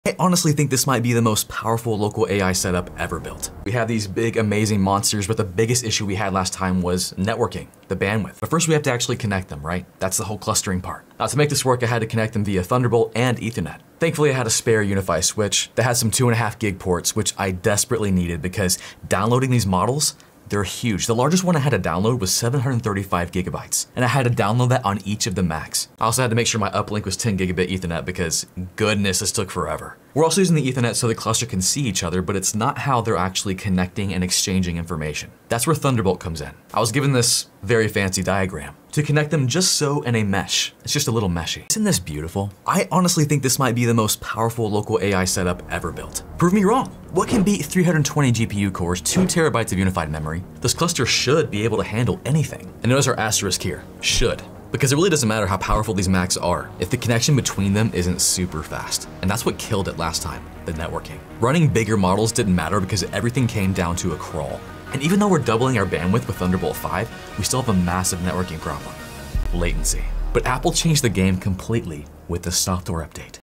The Most Powerful Local AI Ever Built? 🤯💻 320 GPU Cores. Is this the ultimate AI cluster ?
Can you build a "Supercomputer" at home? We’re pushing the limits of local AI by clustering 4 M4 Mac Studios to create a monster setup with 320 GPU Cores and 2TB of Unified Memory.

In this video, we dive into the hardware challenges of building a high-performance AI cluster. We’re using Thunderbolt 5 to double the bandwidth and a mesh network to handle massive models (the largest was 735GB!).

Key Highlights:

The Specs: 320 GPU cores & 2 Terabytes of RAM.

The Bottleneck: Why networking and latency almost killed the project.

The Solution: How Thunderbolt 5 and a new Apple software update changed everything.

Original content - @NetworkChuck all credits to him .

Is local AI the future, or is the cloud still king? Let us know what you think of this mesh setup in the comments!